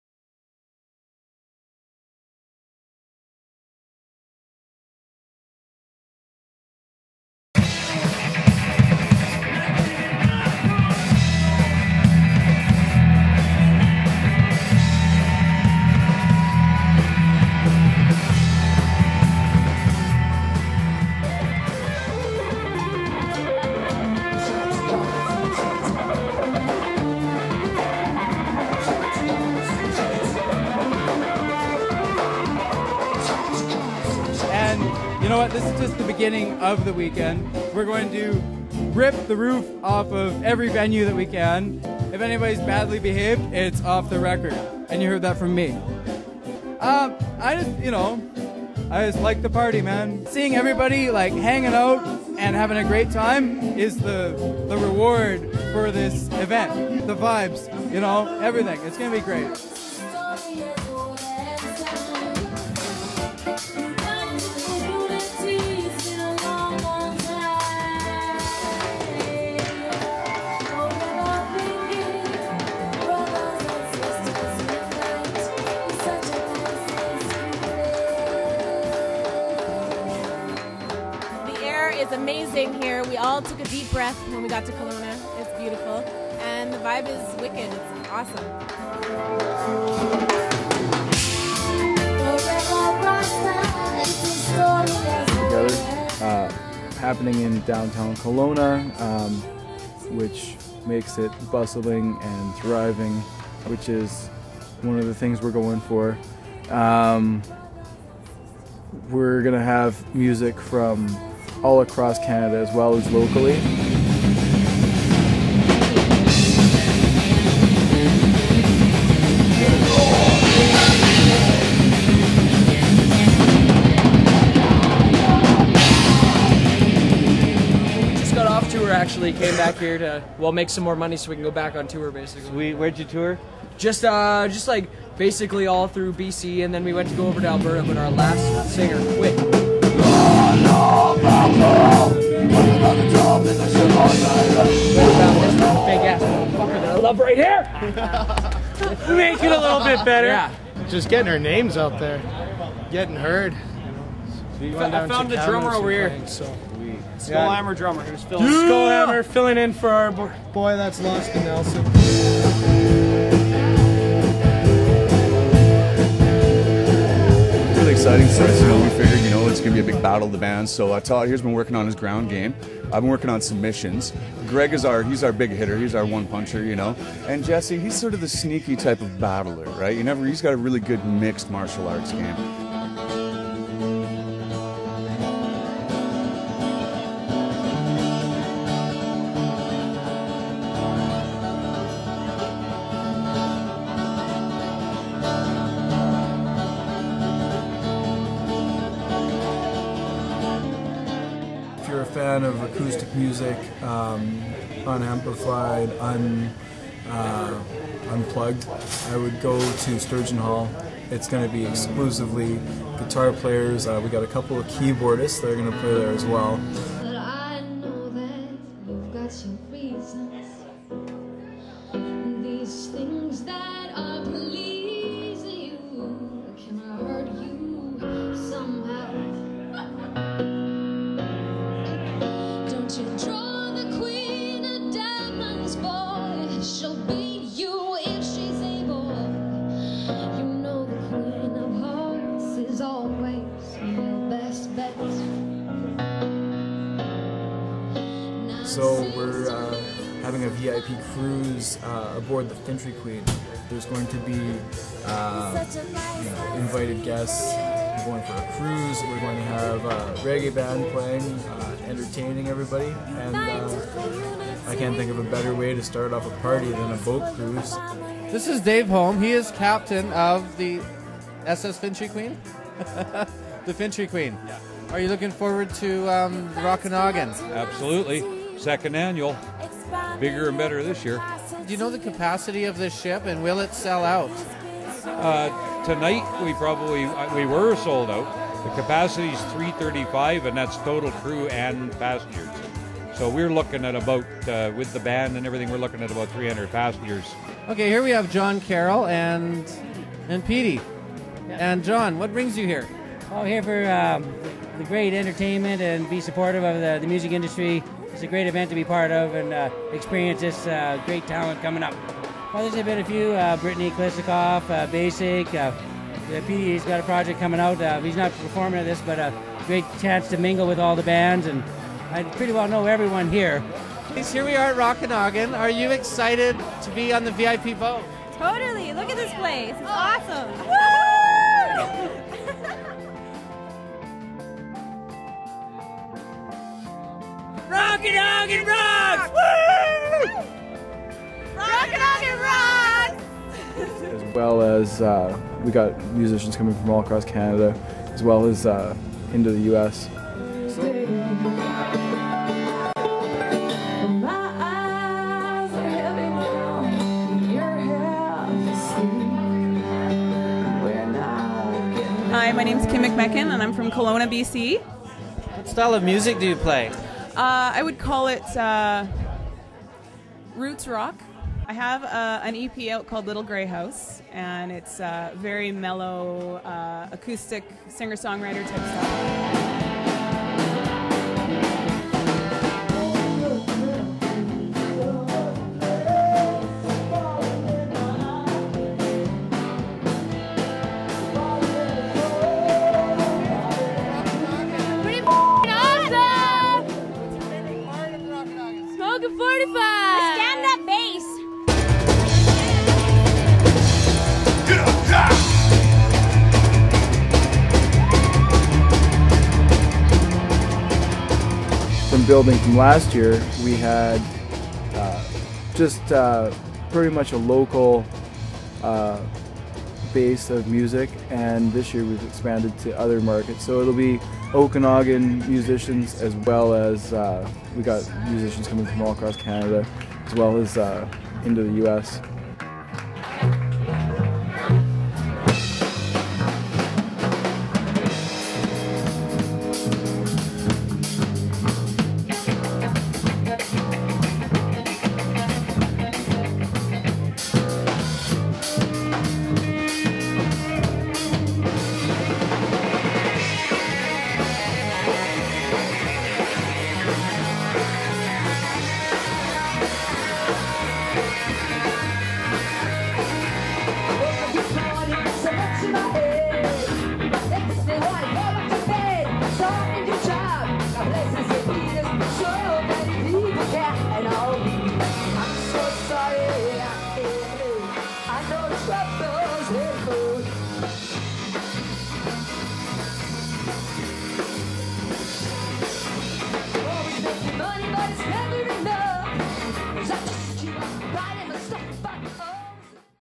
Rockanagan, Kelowna 2008
Kelowna, British Columbia's very own Rock 'n Roll Music Festival. Bands from all across Canada, the U.S.A. and, of course, the Okanagan Valley play a frenzie of metal, reggae, pop, hill billy, you name it. This is where the sun shines and the people rock to great music! Every September. Check it out!